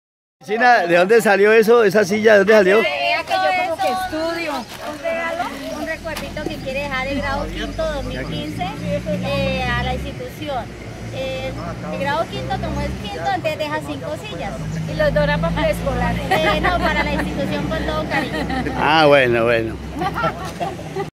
Grados 5o Santa Teresa de P. 2.015
Regalan al colegio bancas, para mayor comodidad de los niños.
                         02-dic-2.015